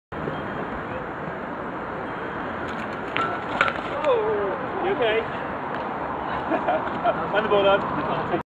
Mind the bollard!
However gentle the cycling pace was, I couldn't help having a little chuckle at this guy colliding with a bollard because he was using his iPhone. Movie originally published by CyclingBristol